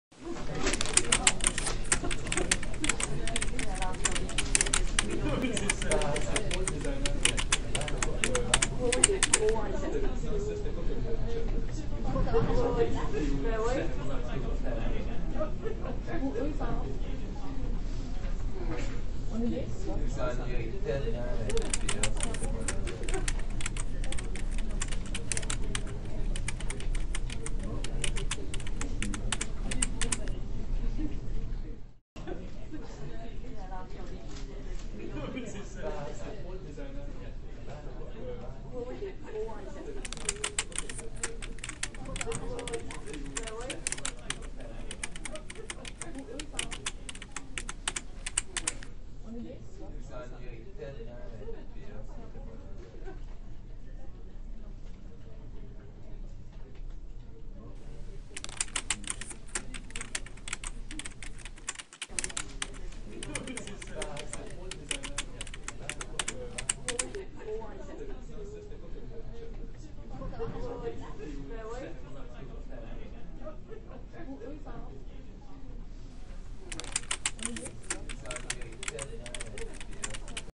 Salle de rédaction de La Presse à Montréal
Une présentation de la salle de rédaction de La Presse à Montréal. C'est là où les journalistes de La Presse conçoivent chaque jour le contenu de l'édition numérique numéro 1 parmi les applications mobiles d'information au Québec.